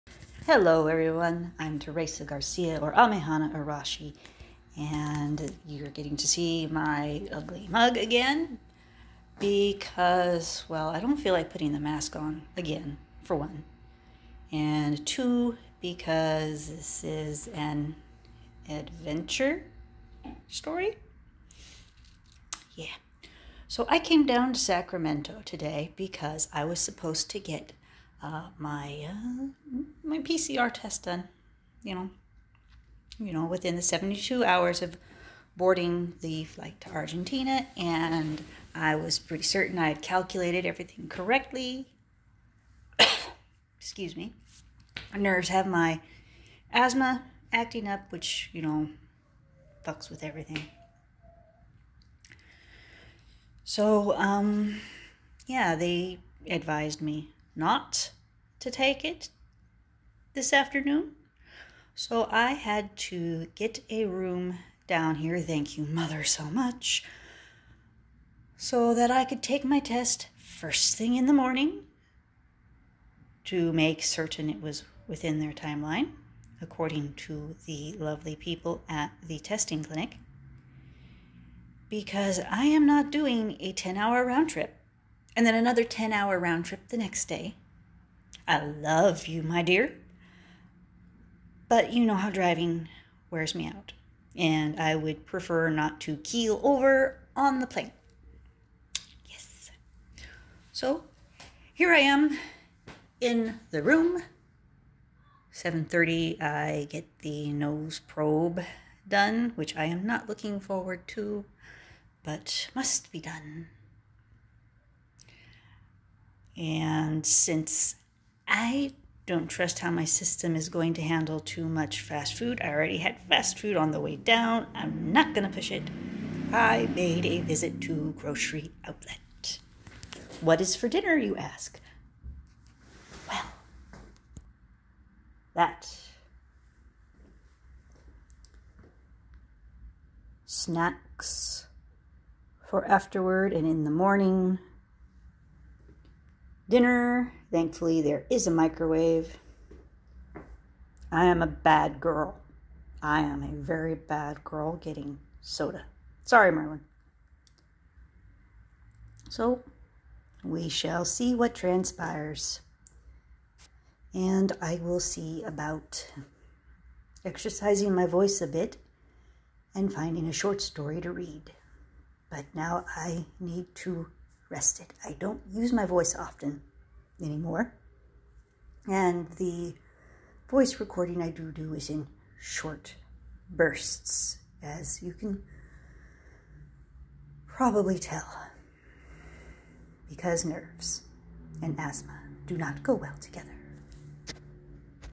Room rambling and food epiphany
I recorded this toward the end of February prior to my flight from Sactown to Argentina on March 1st. I later decided I wasn't going to post it but have since reconsidered since it was such a huge event and since I can't be the only one who hadn't thought of getting dinner from a store while staying in a motel.

I sincerely hope my next visit to another country doesn't entail more all day travel just for one test to be told to wait a few hours. That sucked. The trip was fine, the set up for the trip was hair raising.

As always thank you to my most supportive Patron, mom. Without her the trip would not have been able to happen.